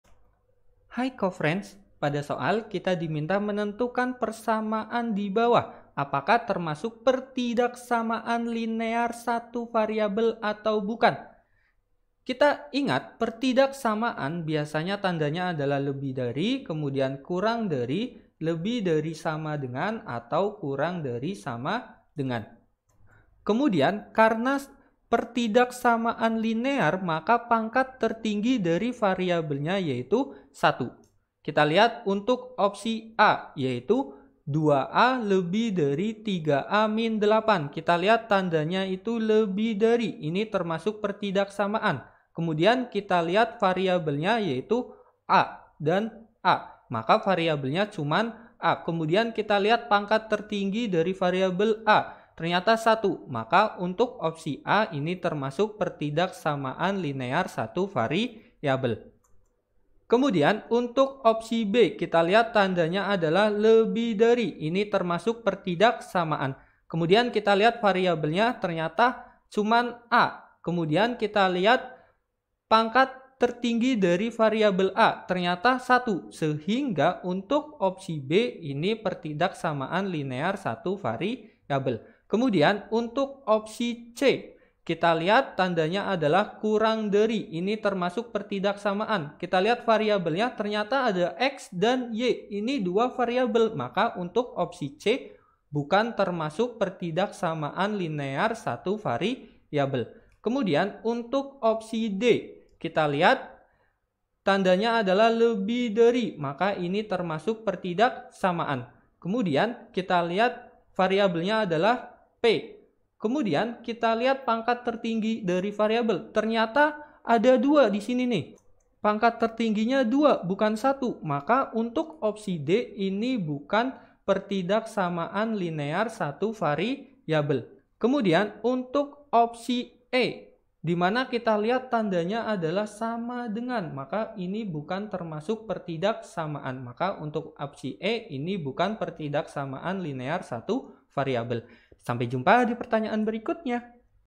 Tentukan persamaan di bawah apakah termasuk PtLSV atau bukan! a. 2a &gt; 3a - 8 b. 2a &gt; 4(3a -...
Tentukan persamaan di bawah apakah termasuk PtLSV atau bukan! a. 2a &gt; 3a - 8 b. 2a &gt; 4(3a - 5) c. 4x &lt; 2y - 4 d. -2p^2 &gt; 3p - 10 e. 10x = 40

Topik atau Materi: Menyelesaikan Pertidaksamaan Linear Satu Variabel (PtLSV) - PERSAMAAN DAN PERTIDAKSAMAAN LINEAR SATU VARIABEL - ALJABAR - Matematika Kelas 7 SMP -

00:00 : Bedah Soal
01:00 : Konsep, rumus dan pengertian Menyelesaikan Pertidaksamaan Linear Satu Variabel (PtLSV)
01:18 : Langkah penyelesaian soal
01:24 : Jawaban akhir
01:54 : Penutup